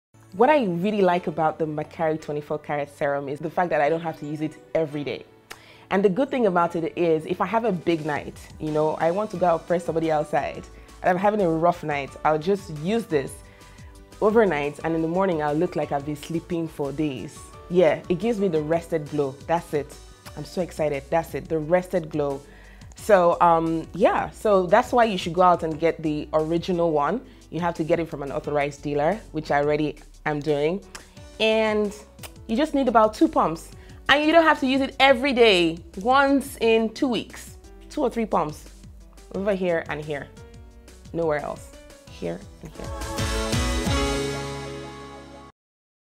Kaylah Oniwo talks about THE MAKARI 24KARAT GOLD SERUM
Watch Kaylah Oniwo share her experience using the Makari 24karat Gold serum aka the booster.